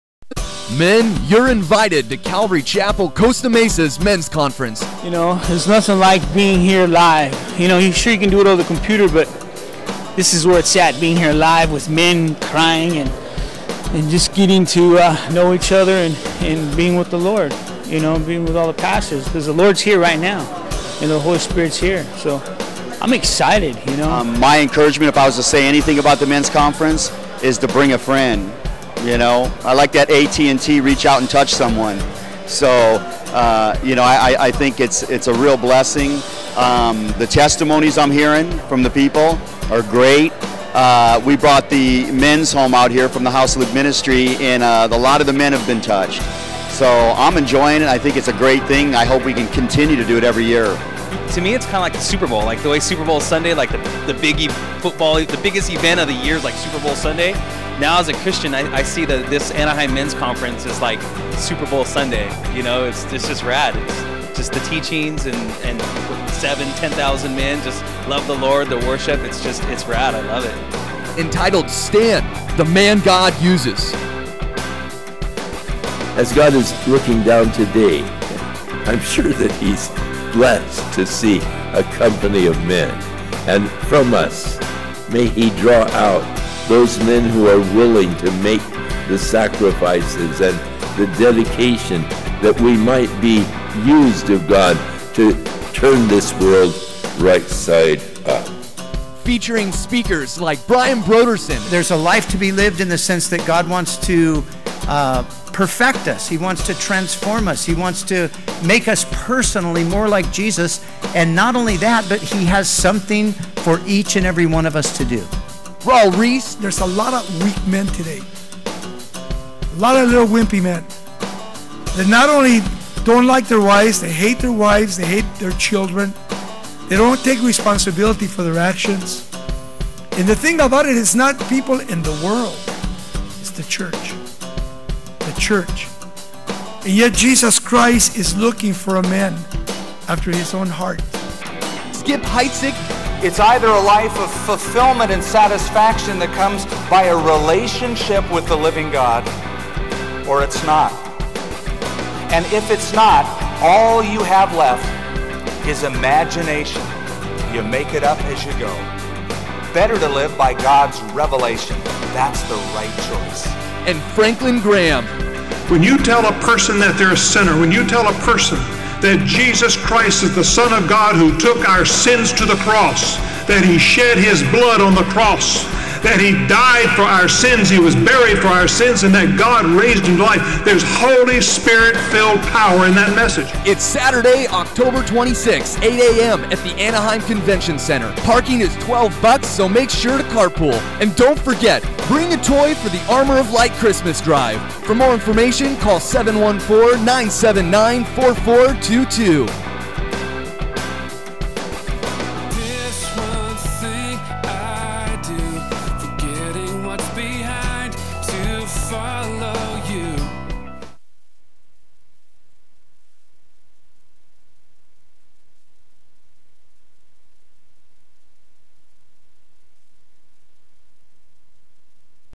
PROMO - 2013 CCCM Men's Conference - Oct 26th at the Anaheim Convention Center
Today - Oct 26, 2013 - is the annual Calvary Chapel Men's Conference in Anaheim, California. This all day event is now being live-streamed @